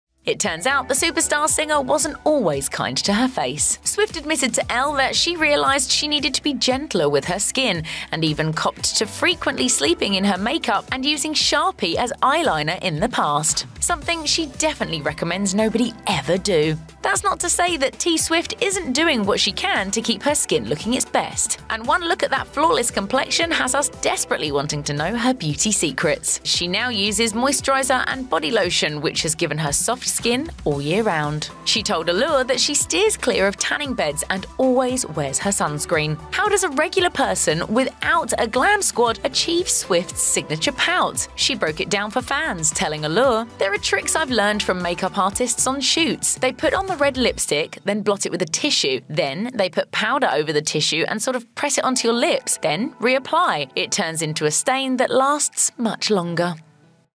Taylor Reveals Her Good And Bad Beauty Secrets
Taylor Swift made some bad makeup decisions growing up that luckily did not affect her long term. Now she has one of the best makeup and no makeup looks in the entertainment industry.